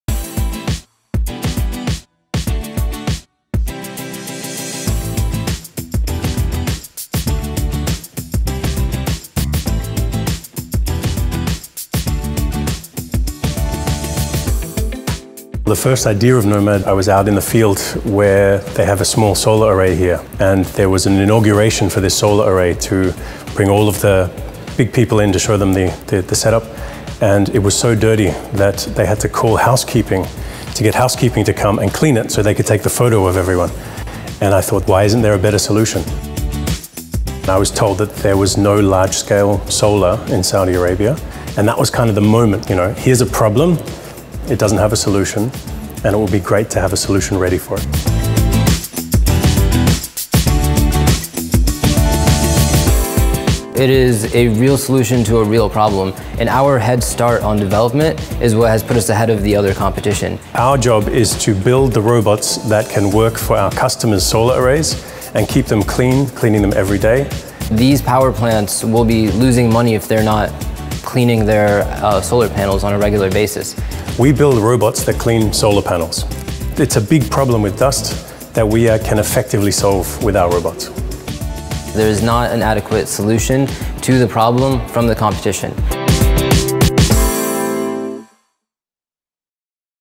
Startup of the Week: revolutionary solutions for the Saudi Arabia’s clean energy sector
Harsh weather can affect the function of Solar panels in the desert, this is how a Saudi startup is using robots to keep them clean.